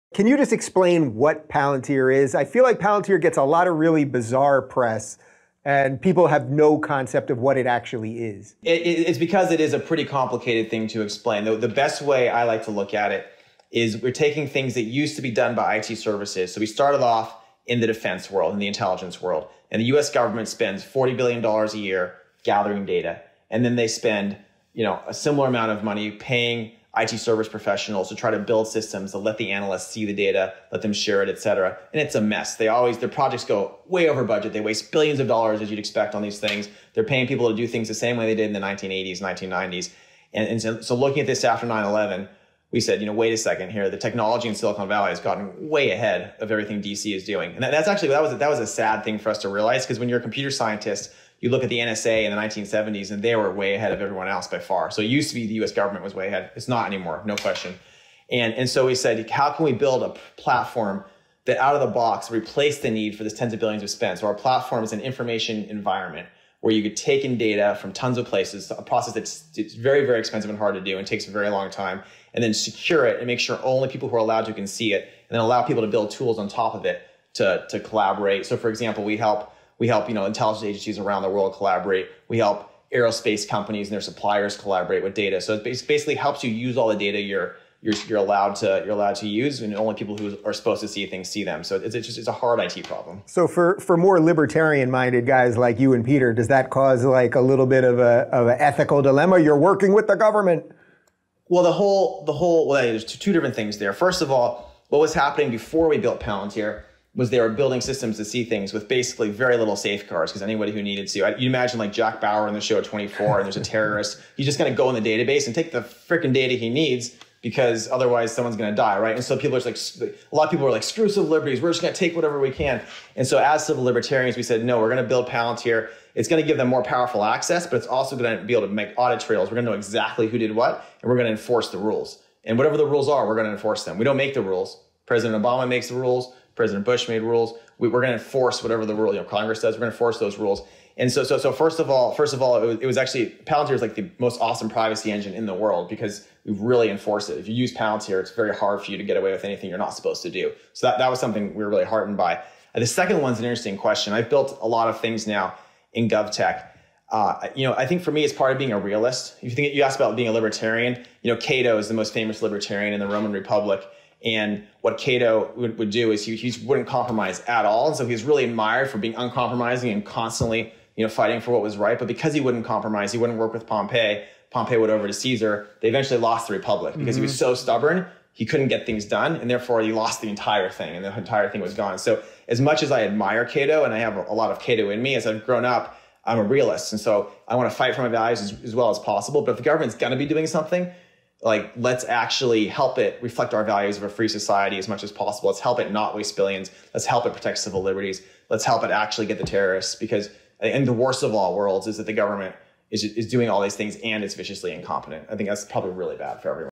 Palantir's $80 Billion Opportunity
Palantir Co-Founder, Joes Lonsdale, comments on how Palantir works and the opportunity that the company has.

- Are you exceptionally knowledgeable about Palantir?

Words that make this video finable.